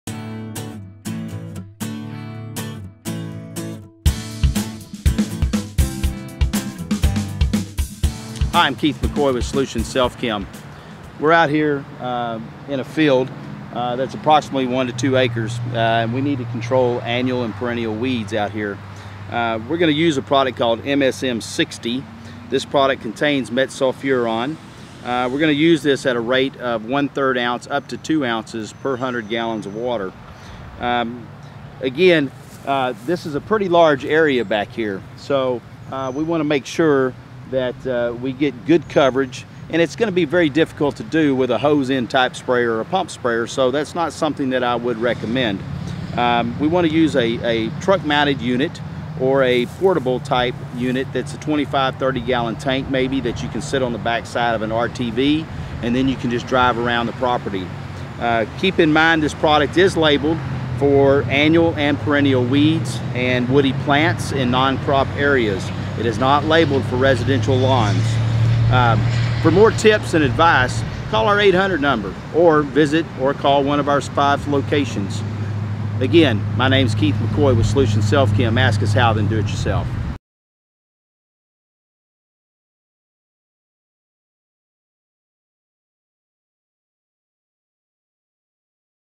MSM 60 DF Herbicide Review Guide
MSM 60 DF herbicide (generic manor herbicide) is for control of Virginia Buttonweed.